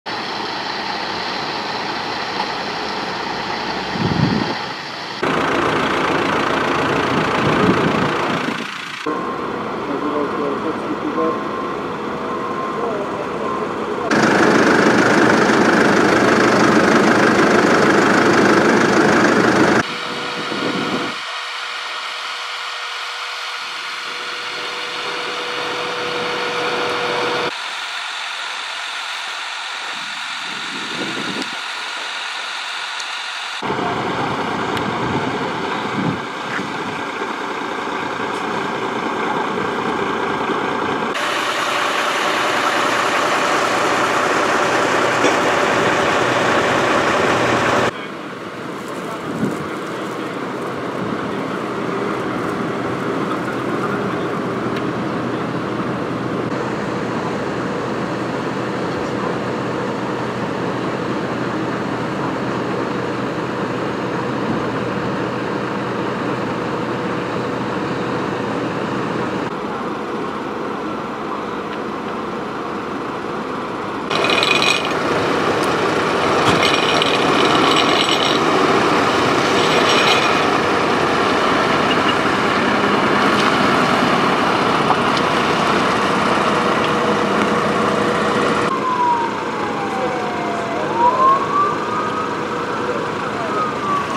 Cisew: Śmiertelny wypadek z udziałem cysterny na 470-tce
Przed godziną 15:00, na drodze wojewódzkiej 470 Turek-Kalisz, doszło do poważnego wypadku drogowego. W wyniku zderzenia peugeota 307 z cysterną transportującą mleko, śmierć poniósł mężczyzna. Trwa akcja ratownicza, a trasa jest obecnie całkowicie nieprzejezdna.